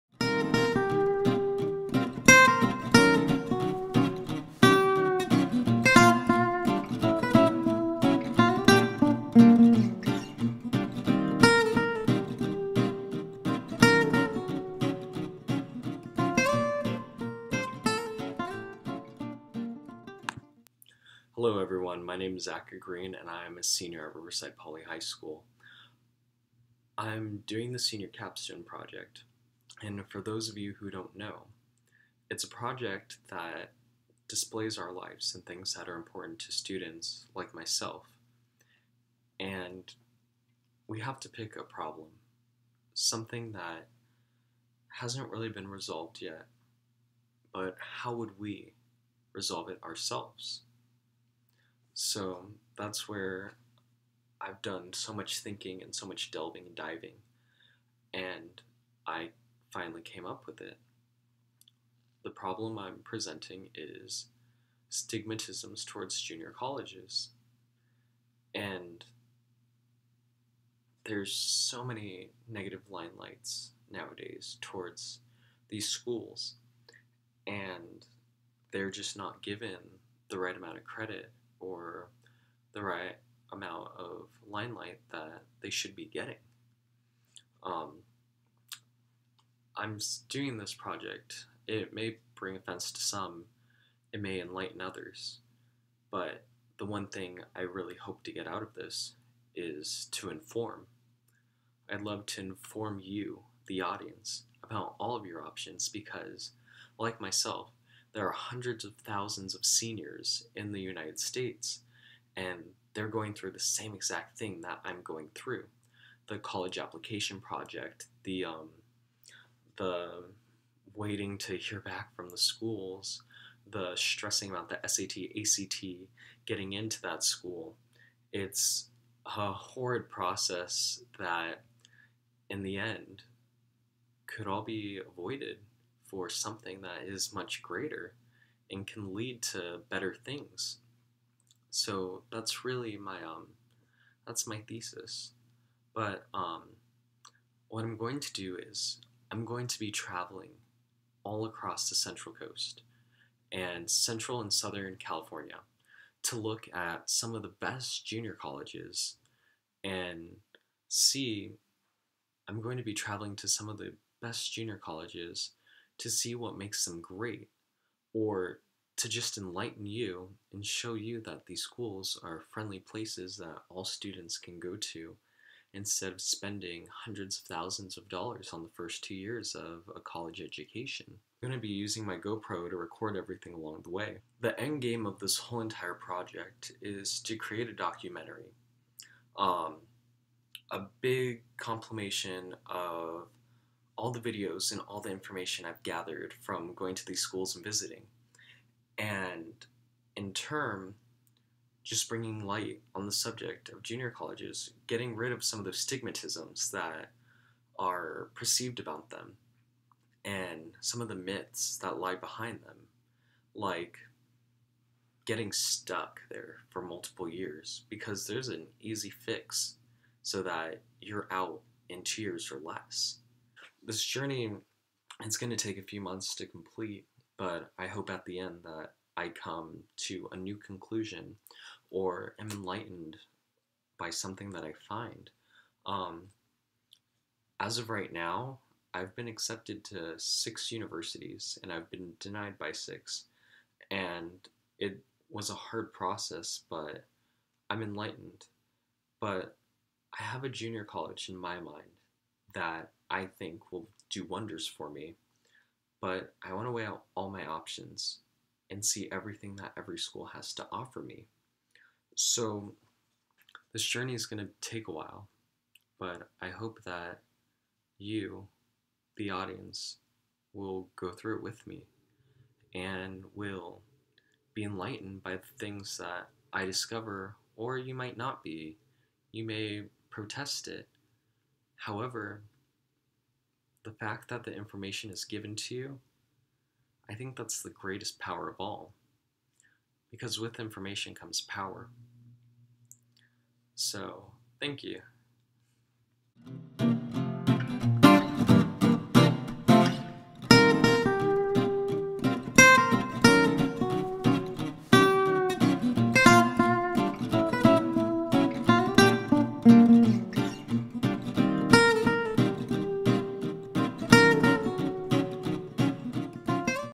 Zachary Green Introduction Vlog 1: "Eww Junior Colleges..." But Why???
For my senior project, I am hoping to inform my audience about the awesome opportunities that junior colleges offer to all students. I hope to bring more options to a student that is looking at colleges, that way they are looking at all of the opportunities available to them. This is just an intro and background into my documentary that will be released in a few months.